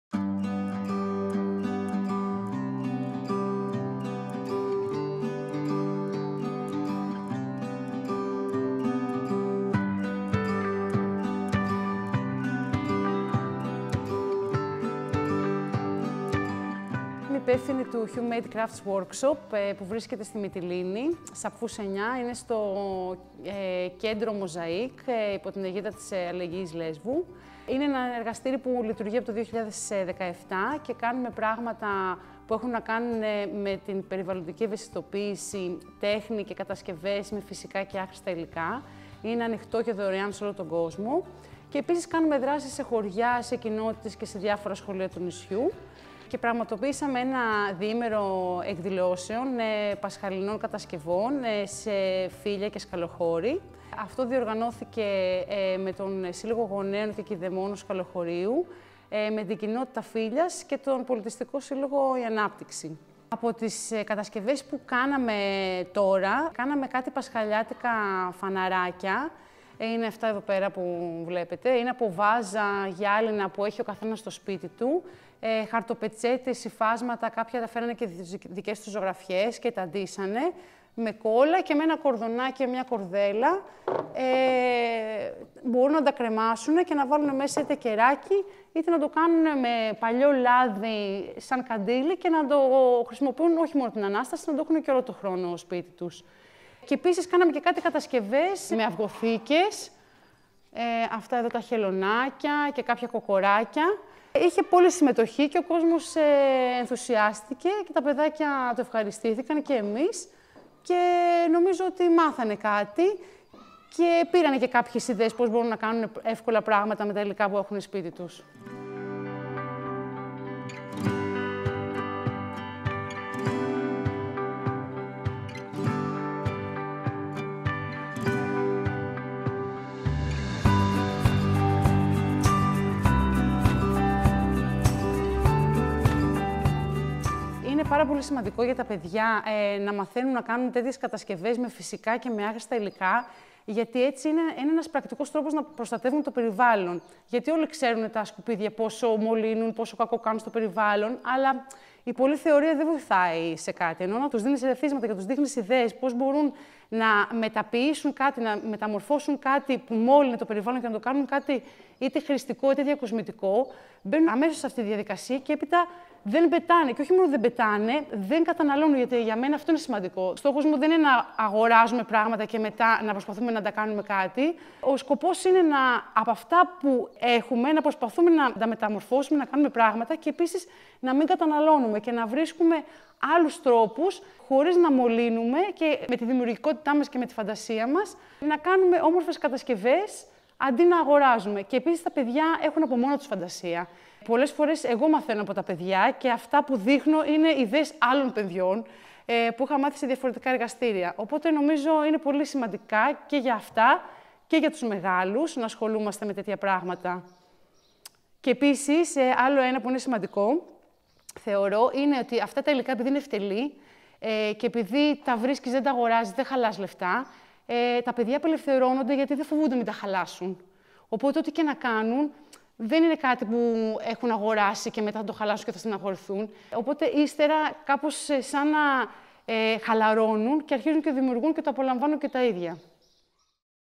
Τα πρώτα «ΦιλιοΣκαλοχωρίτικα» είναι γεγονός, με την Γιούλα Κουτσουμπού
Μαϊάκη: «Η πρώτη από κοινού εκδήλωση  Φίλιας & Σκαλοχωρίου για τα παιδιά»